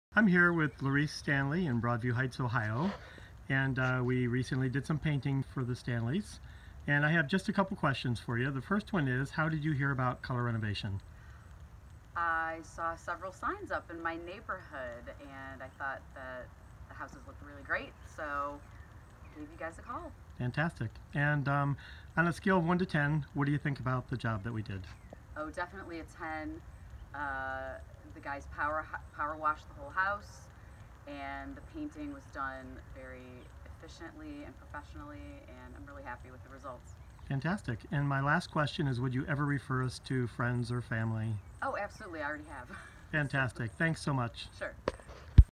House painting Broadview Heights
Lorece shares her experience with Color Renovation. We painted the exterior of her home in Broadview Heights